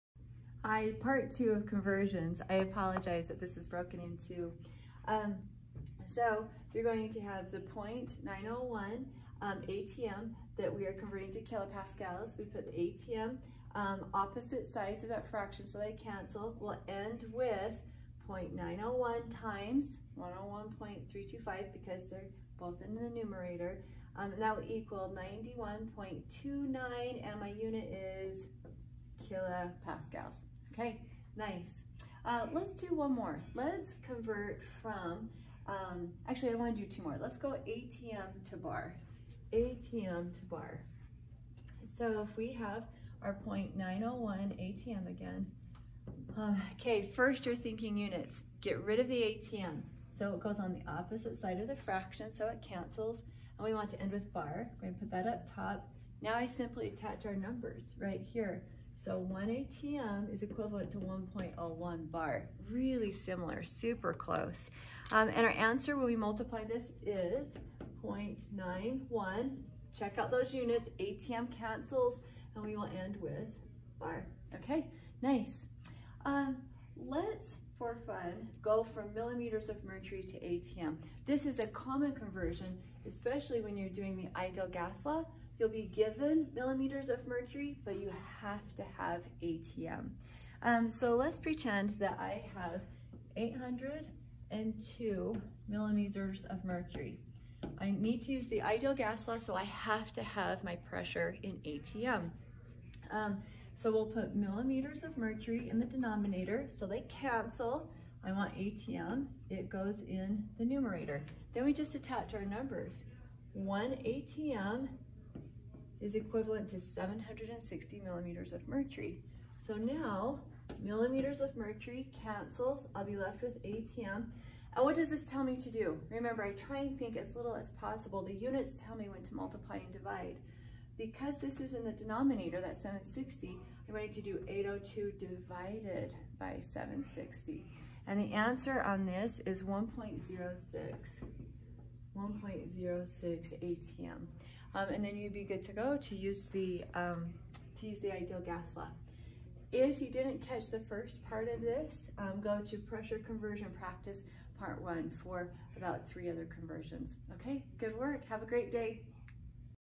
Pressure Conversion Practice Part II
Examples converting between pressure units.